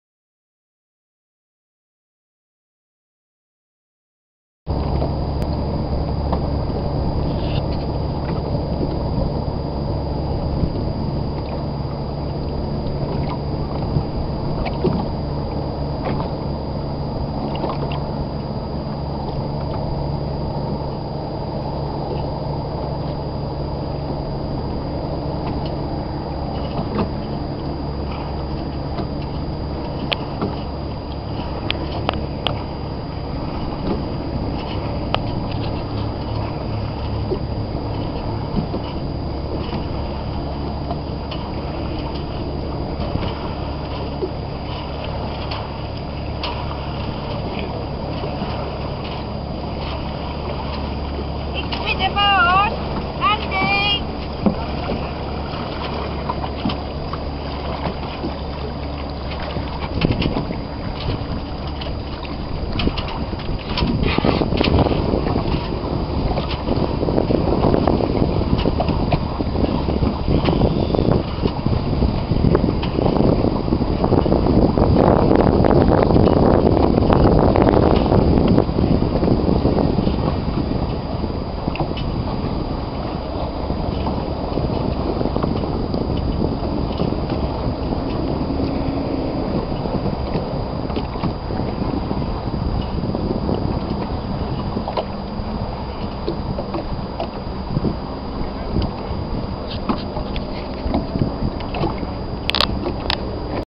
Woods Surfsong - Alley Cat
Alley Cat sailing 26/05/11, San Antonio Bay - Ibiza
Skipper: Barnicle Bill, Crew: Cj